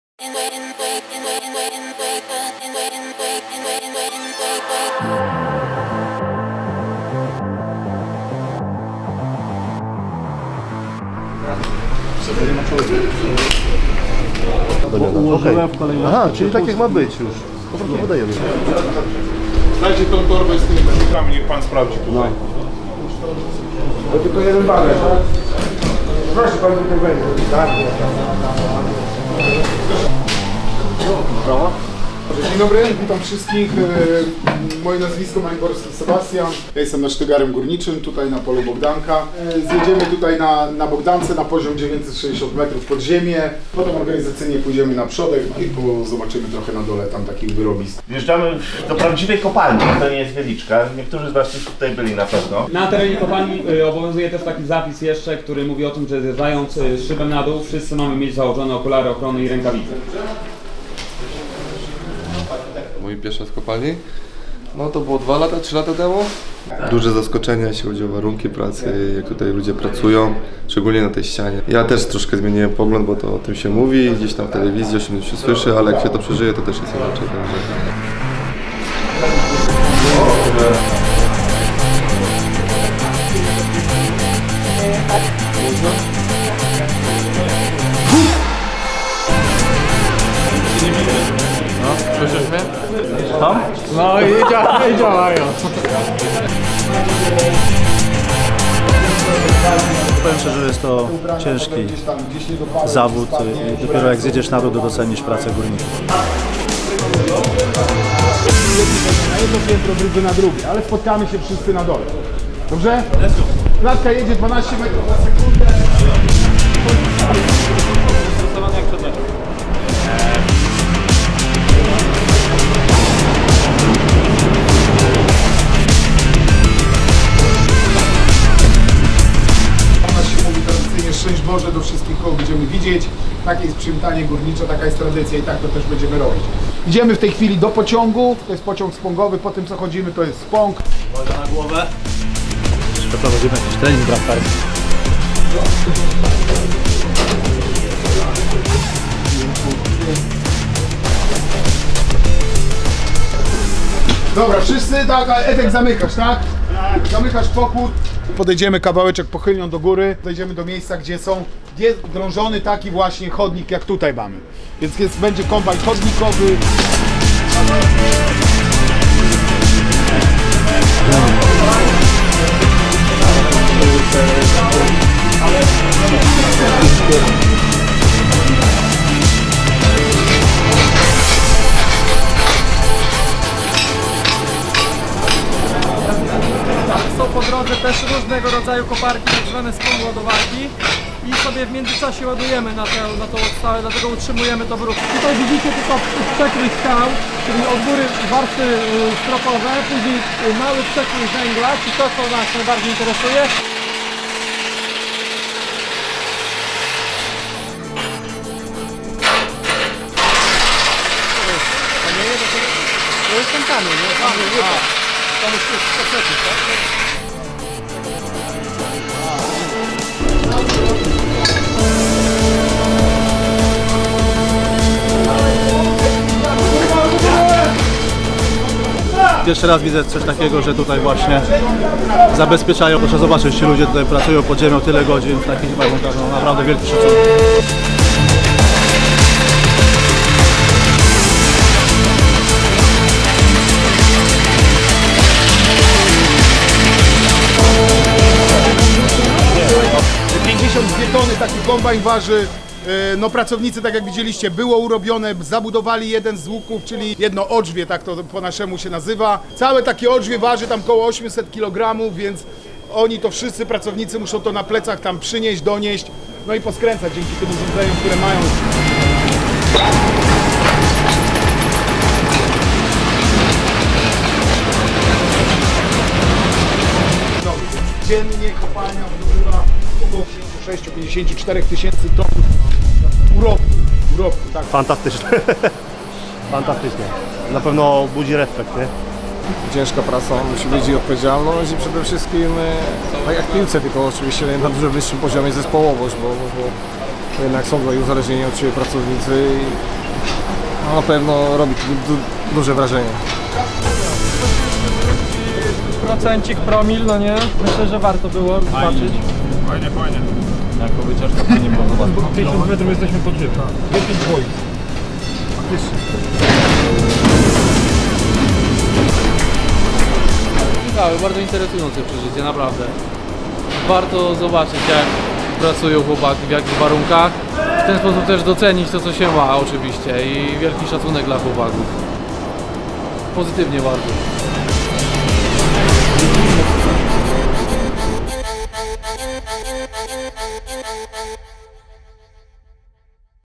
Górnik Łęczna w Bogdance
Zjazd piłkarzy Górnika Łęczna do wyrobisk dołowych w Polu Bogdanka w LW Bogdanka.